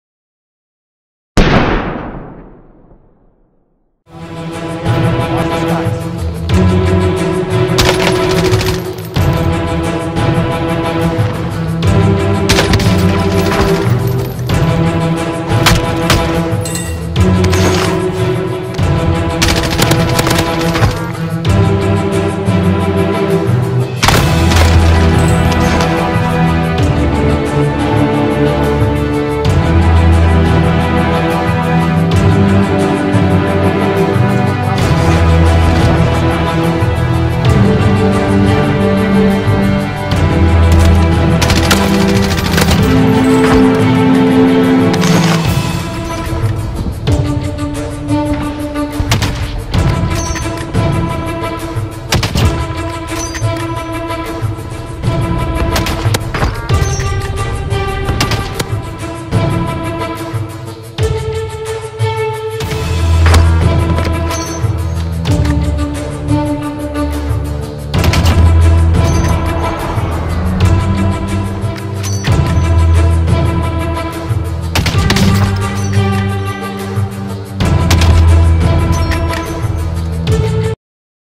My first three kills were the same guy
Mostly sniping in this one.  My first three kills were the same guy.  He tried to escape the last time but he was hunted down like a dog.  Not gonna lie, it felt good.